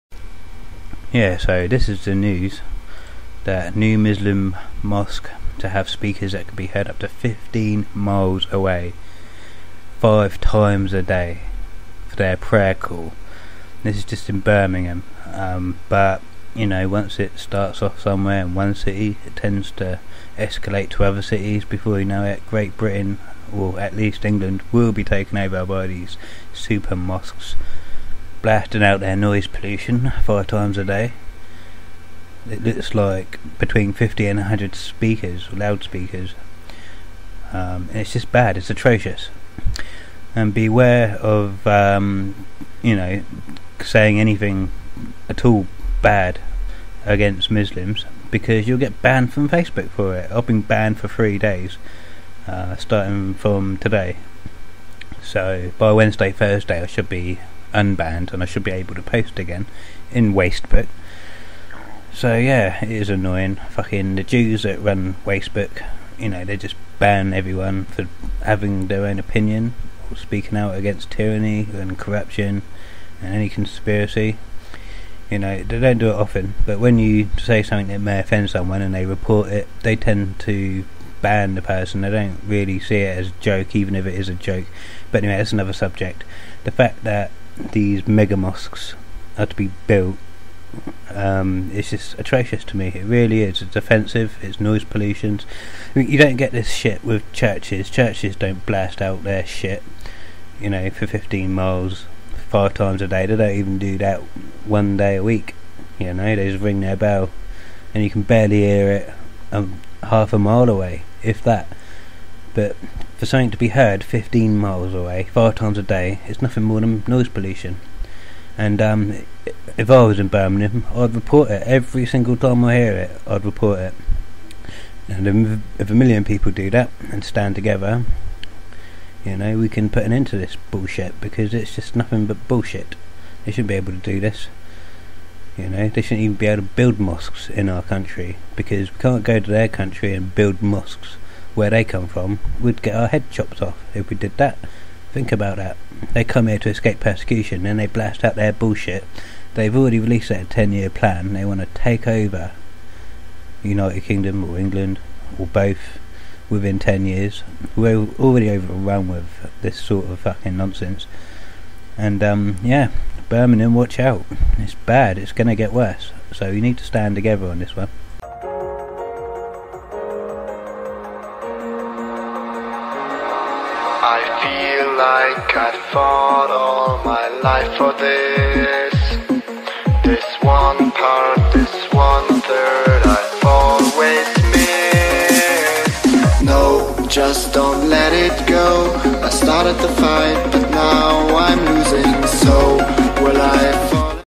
My Thoughts on This Bad News: Super Mosque 2 have Speakers will B Heard up to 15 Miles
news.bfnn.co.uk/new-muslim-mosque-to-have-speakers-that-can-be-heard-up-to-15-miles-away

Muslim takeover of Britain!